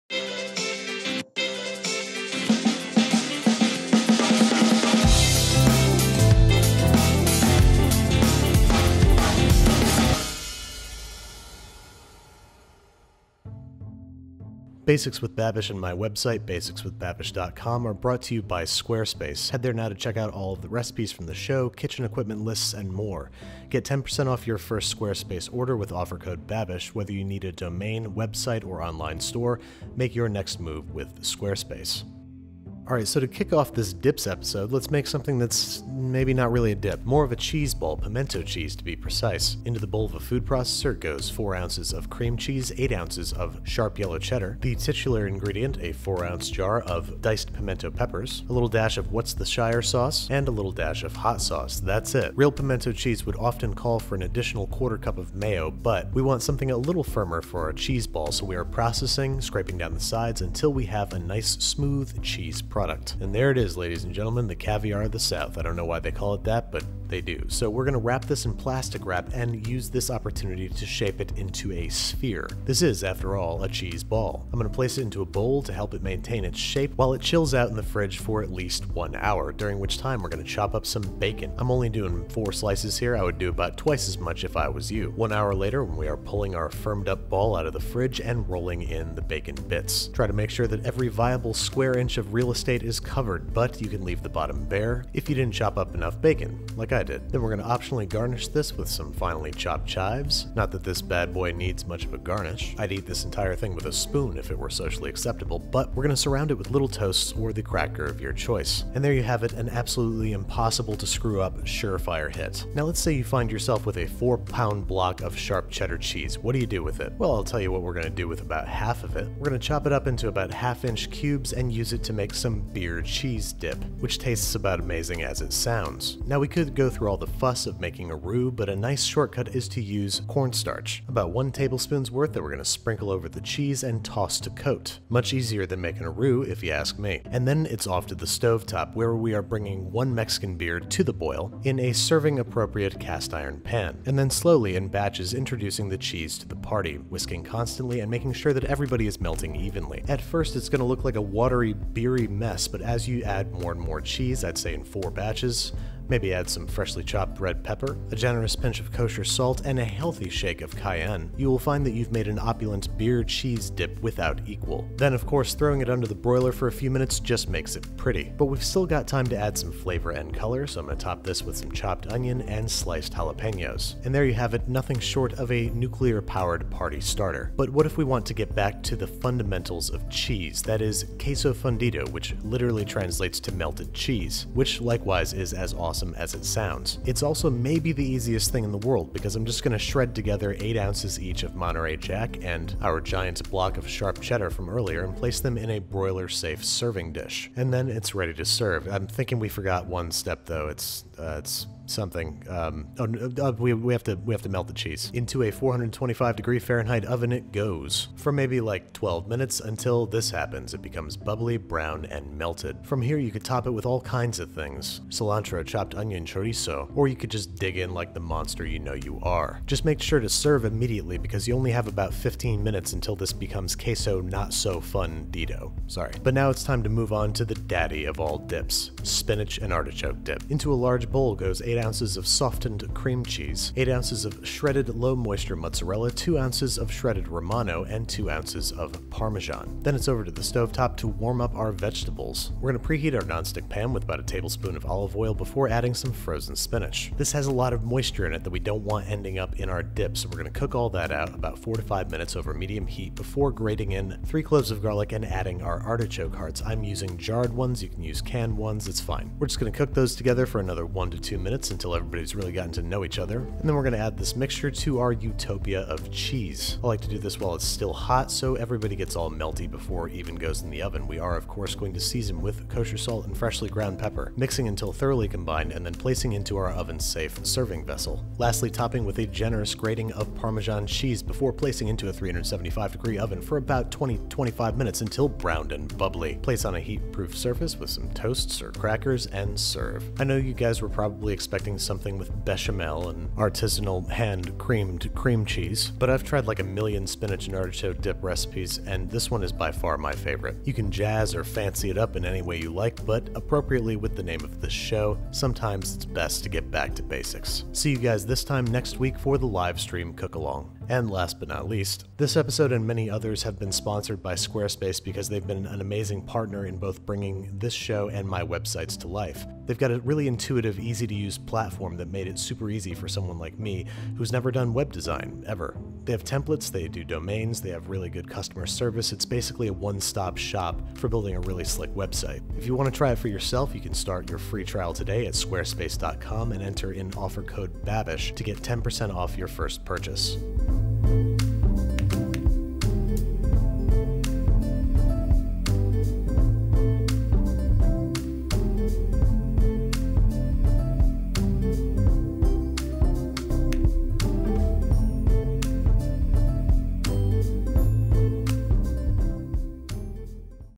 Cheese Dips | Basics with Babish
It’s a societal responsibility to, at one point or another, provide some kind of dip for a group of hungry party goers - and we’ve all known the anguish of seeing our painstakingly-prepared party provisions go virtually untouched, while Jeff from down the street practically has his bowl licked clean.  Well, now it’s your turn - let’s get down to Basics.

Ingredients & Shopping List

+ For the Pimento Cheese Ball:
4 ounces cream cheese
8 ounces sharp yellow cheddar
4 ounce jar of diced pimento peppers
Dash of Worcestershire sauce
Dash of hot sauce
6-8 slices bacon, cooked and chopped
Chives, finely chopped (for garnish)
Toast or crackers (for serving)

+ For the Beer Cheese Dip:
2 lbs sharp cheddar cheese, cut into 1/2 inch cubes
1 Tbsp corn starch
1 can Mexican beer (like Modelo)
1/4 cup chopped red pepper
Kosher salt
Freshly ground pepper
1/4 cup chopped red onion
1 jalapeno, sliced
Tortilla chips (for serving)

+ For the Queso Fundido:
8 ounces jack cheese, grated
8 ounces sharp cheddar cheese, grated

+ For the Spinach and Artichoke Dip:
8 ounces cream cheese, softened
8 ounces low moisture mozzarella, grated
2 ounces romano cheese, grated
2 ounces parmesan, grated
1 Tbsp olive oil
2 cups frozen spinach
3 cloves garlic, grated
1/2 cup artichoke hearts
Kosher salt
Freshly ground pepper

+ Special equipment:
Food processor
Oven safe skillets, pans, or serving vessels